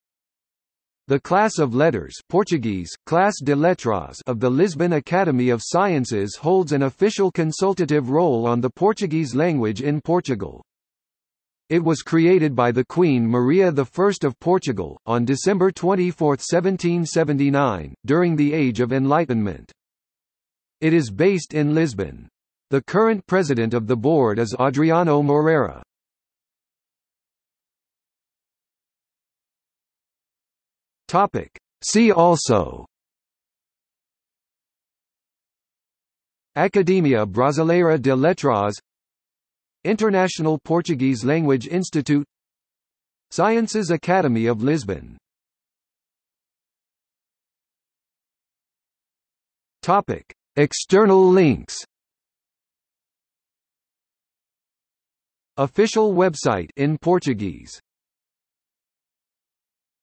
Lisbon Academy of Sciences, Class of Letters | Wikipedia audio article
00:00:27 See also
00:00:46 External links

- Socrates

SUMMARY
The Class of Letters (Portuguese: Classe de Letras) of the Lisbon Academy of Sciences holds an official consultative role on the Portuguese language in Portugal.
It was created by the queen Maria I of Portugal, on December 24, 1779, during the Age of Enlightenment.
It is based in Lisbon. The current president of the board is Adriano Moreira.